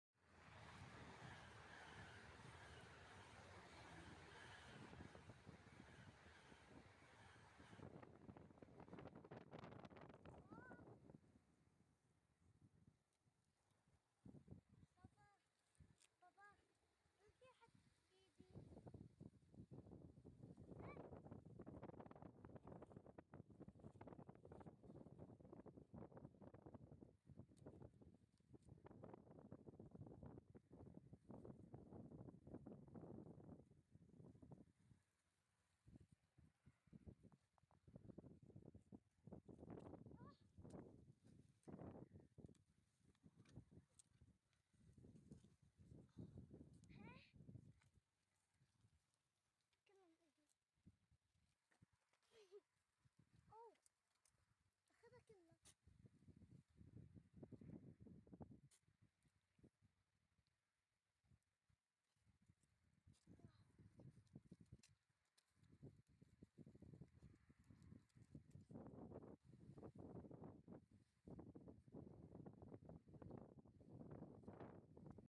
Hungry deer
I photograph animals with my camera, I photograph all domestic and wild animals

The gazelle is a mammal. Deer are found in deserts, grasslands, and Africa.  Also in southwest and central Asia and the Indian subcontinent.  They like to live in flocks and feed on herbs.  It is not a huge animal